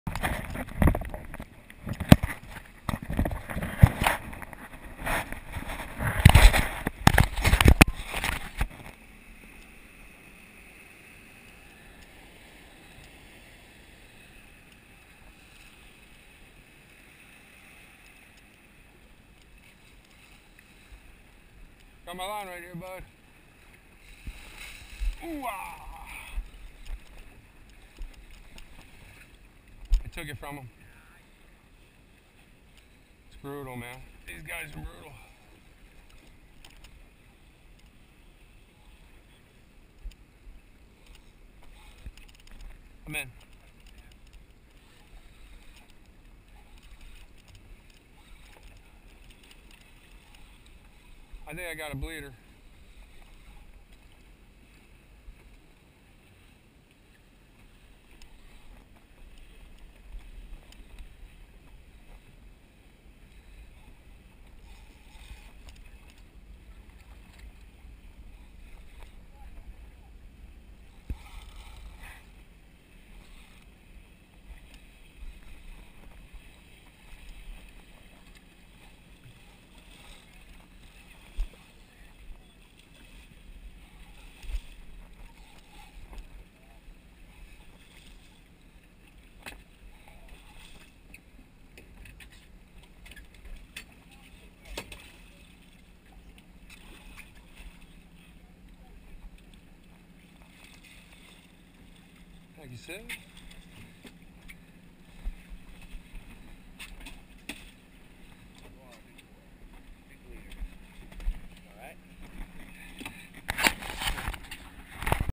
Striper fishing w/ live bunker
11/5/15 fishing off of Island Beach State Park, NJ w/ my buddy Mike " 2  Live Crew"